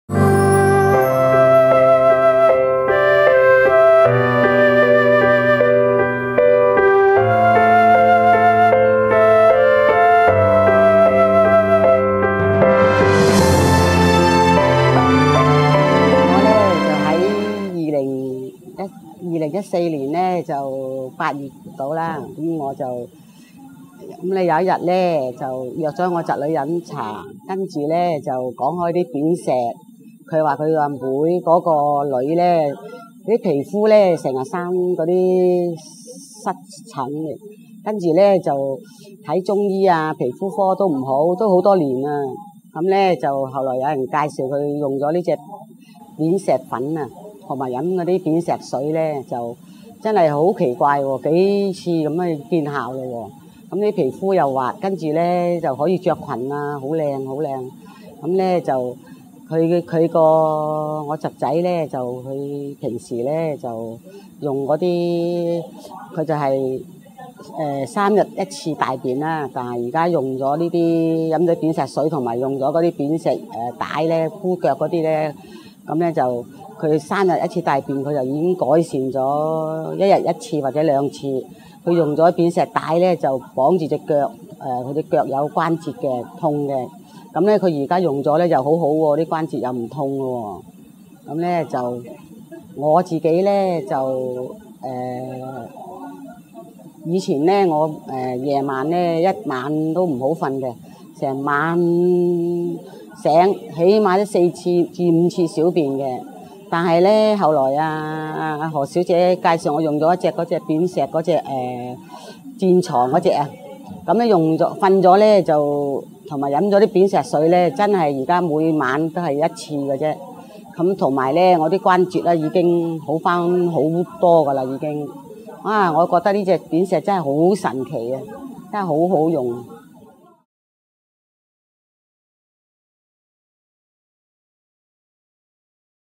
砭石用家訪談錄2
砭石的神奇療效，讓鍾女士興奮的講述她的故事。